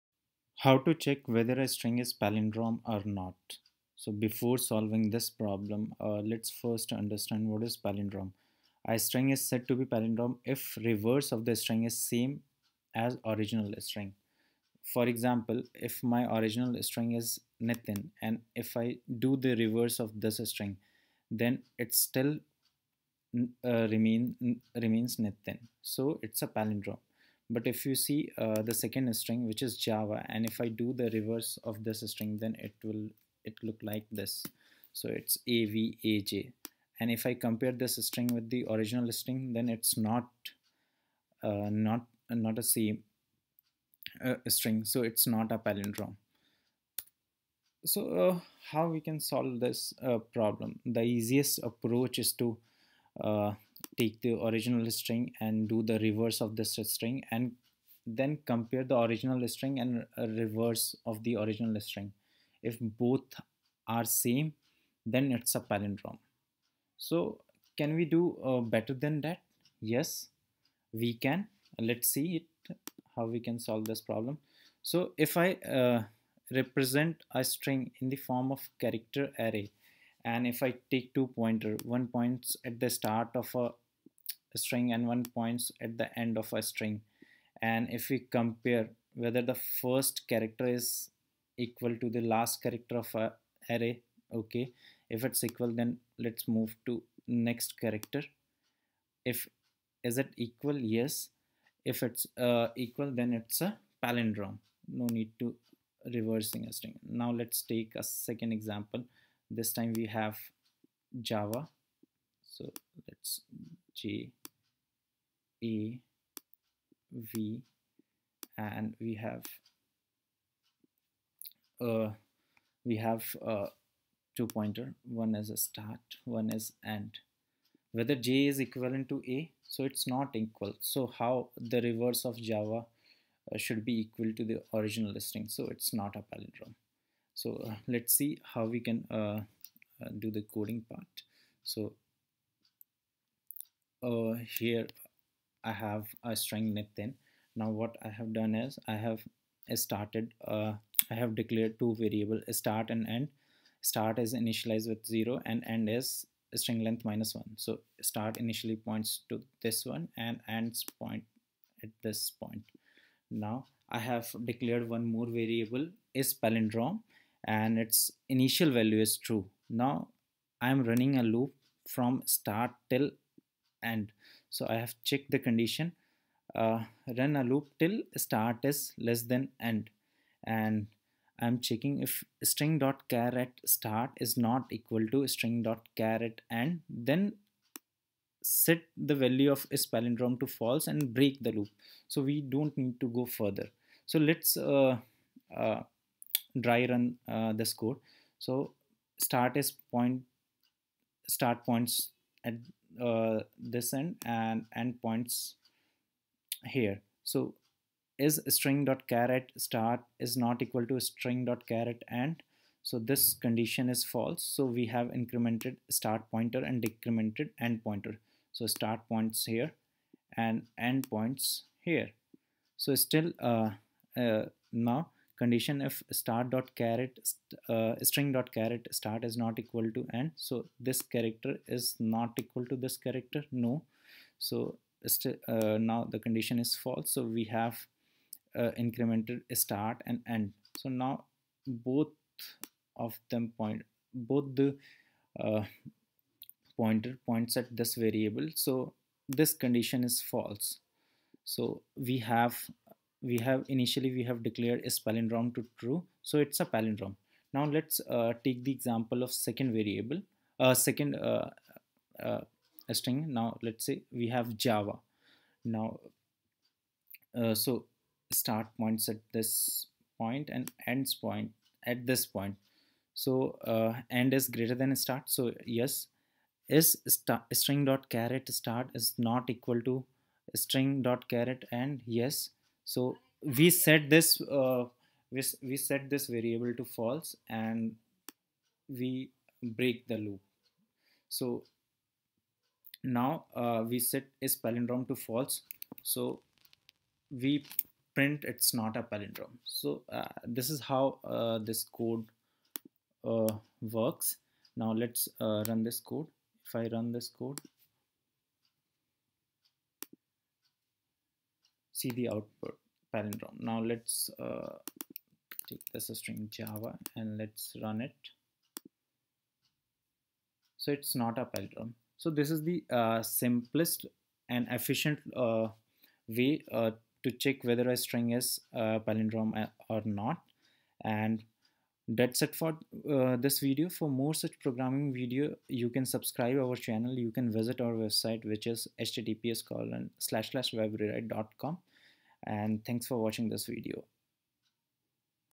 Java Program to Check String is a Palindrome or Not
In this tutorial, I have explained java program to check whether a string is palindrome or not.